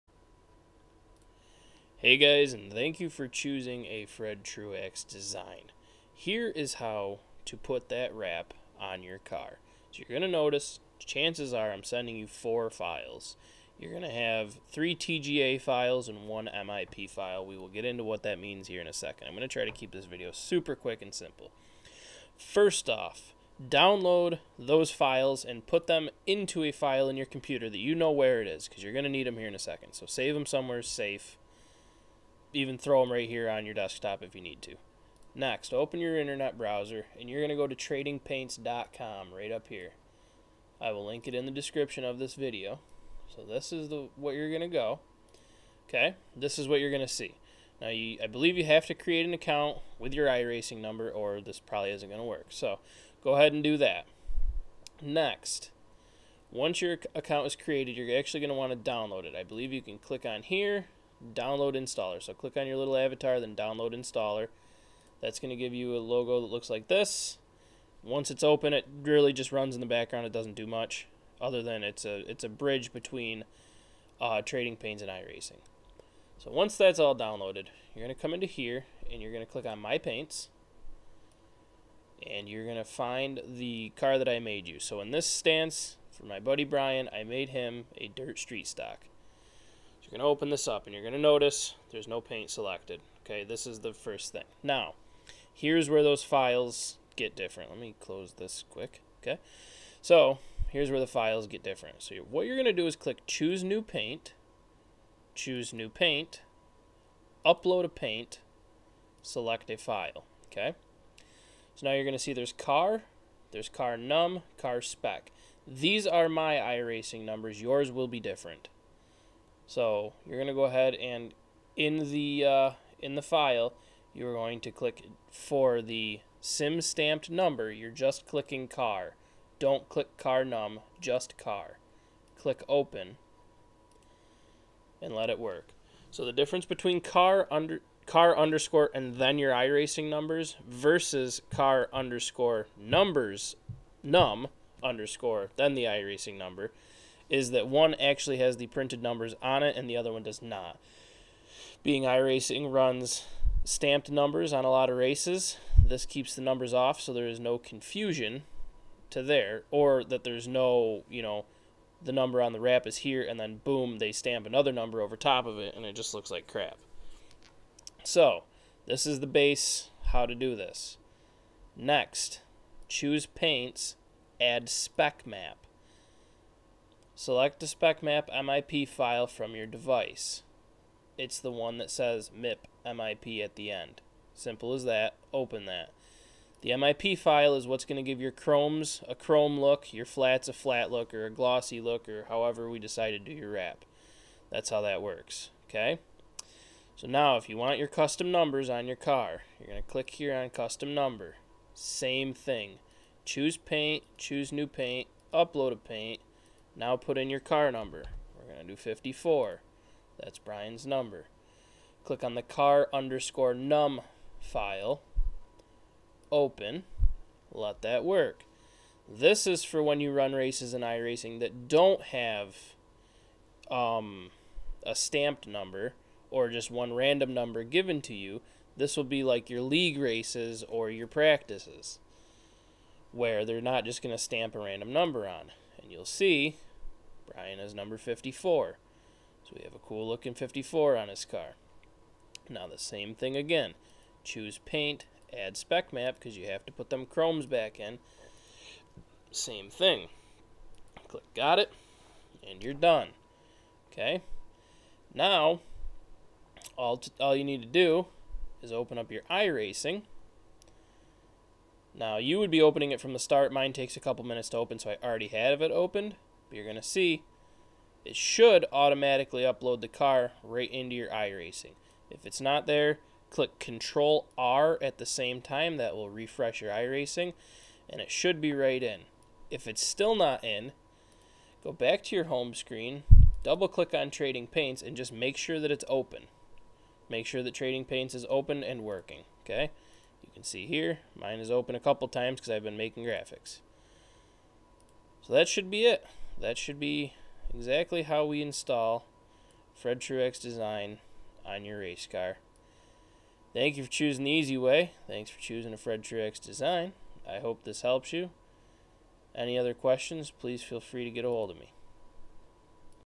How to install I racing Wraps to Trading paints!
a quick video on installing graphics through Trading paints to IRACING